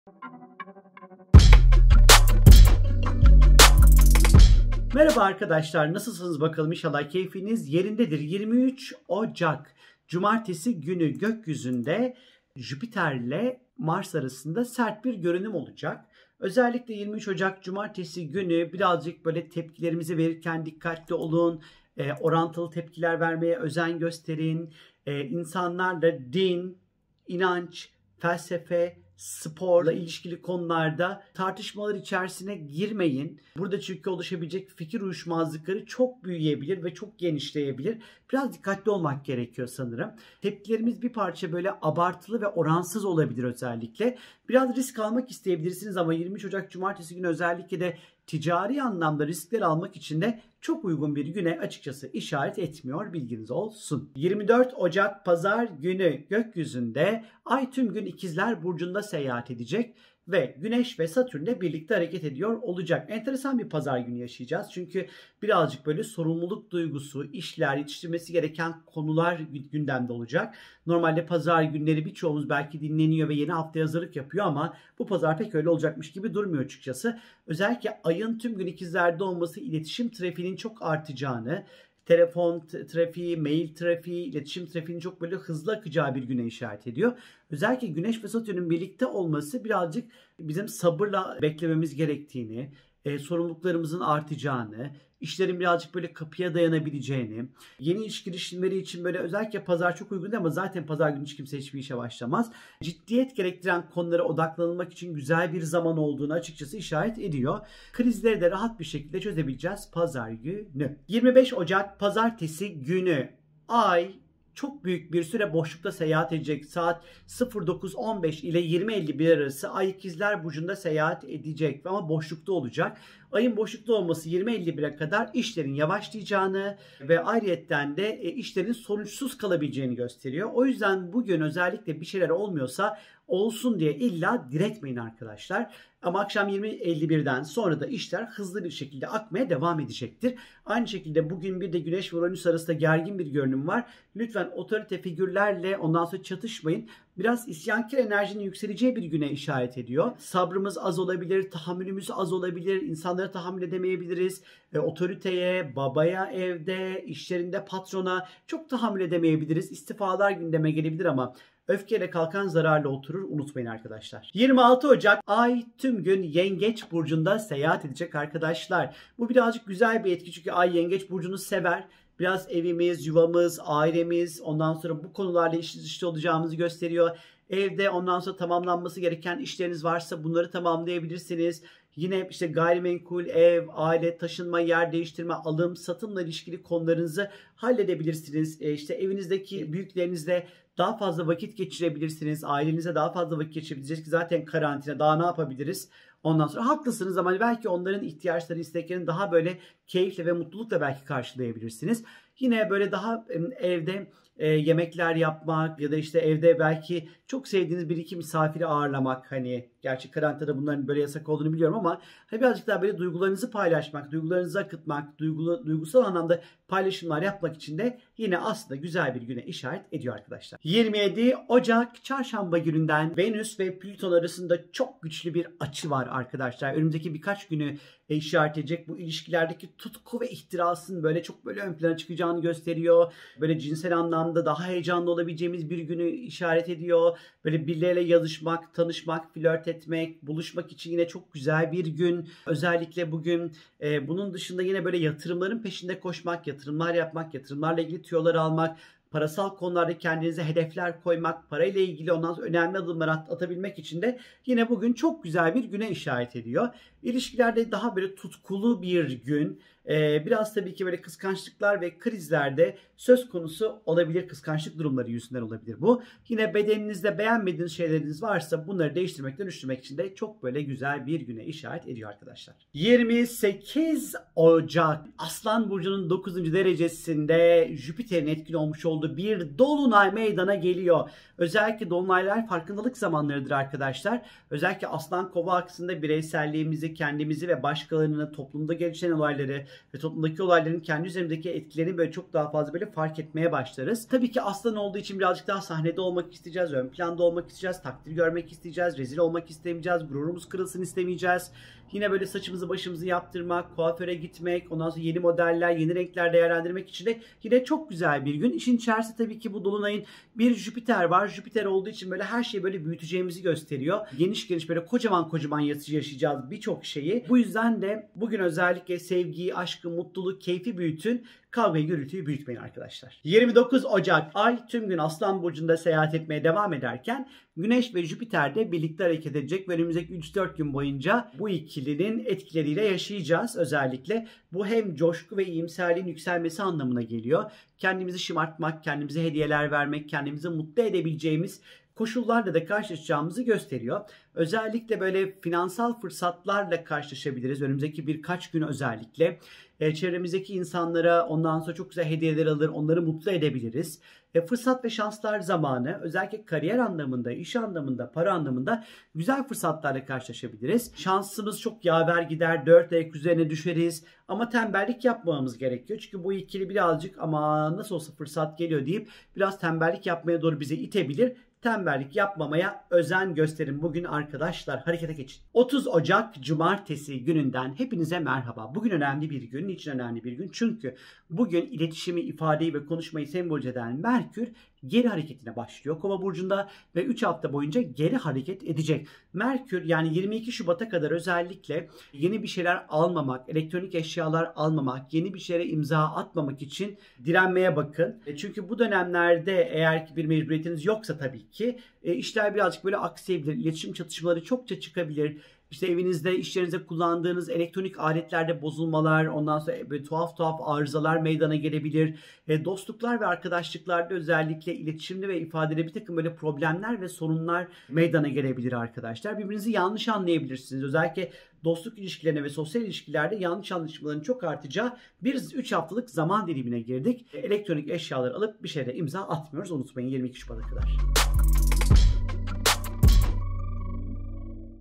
Dinçer Güner'den 23-30 Ocak Arası Burç Yorumları
2021'e dair öngörüleri, isabetli tahminleriyle herkesi şaşırtan astrolog Dinçer Güner artık Hürriyet’in hafta sonu eklerinde. Güner, 23-30 Ocak tarihlerini kapsayan astroloji yorumlarıyla karşınızda!